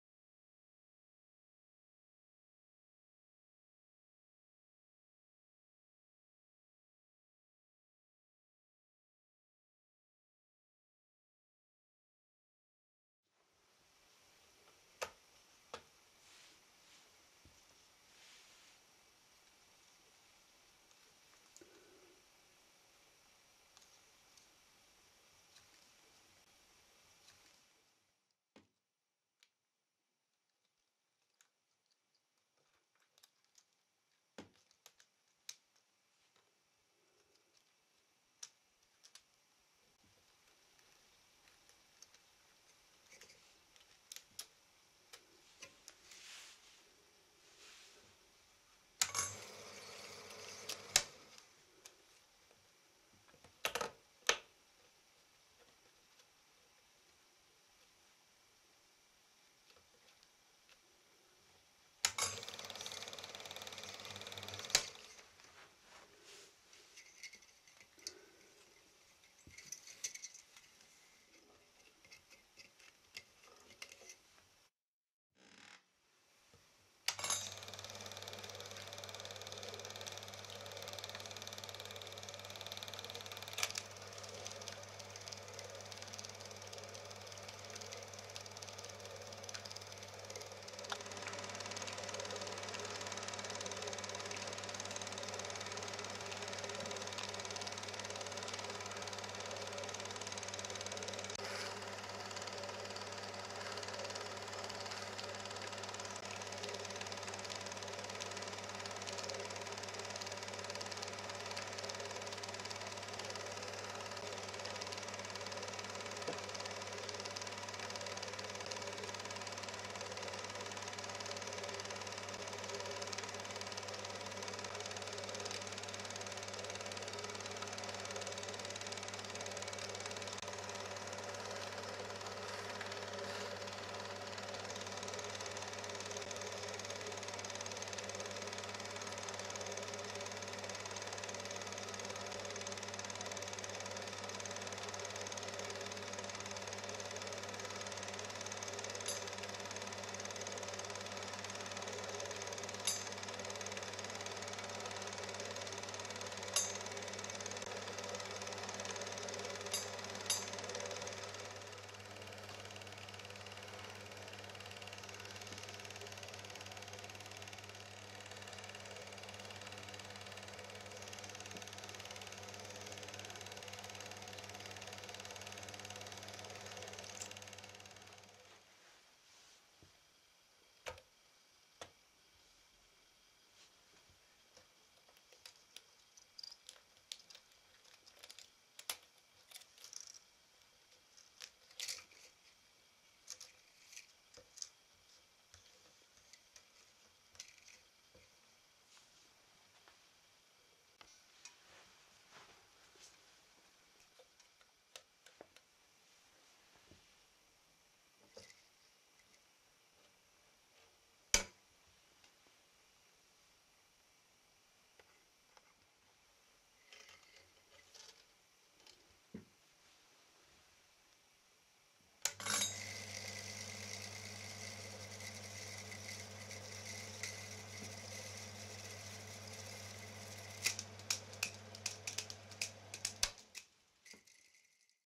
BAUER T10 S
Dieser BAUER T10  S ist die Weiterentwicklung der in den 60 er Jahren erfolgreichen T10 - Normal 8 - Projektoren.
Der "T10  S" unterscheidet sich vom Vorgänger - Typ "T10  R"  dadurch, dass ein "Impulsgeber" eingebaut ist, um ein Steuer-Signal auf einen modifizierten MC-Kassettenrekorder bzw. auf ein Tonbandgerät mit aufzuzeichnen. Auf diese Weise konnten später Projektor und Tongerät "synchron" miteinander laufen, was  eine "Synchrone"  Tonwiedergabe zum Filmgeschehen erleichterte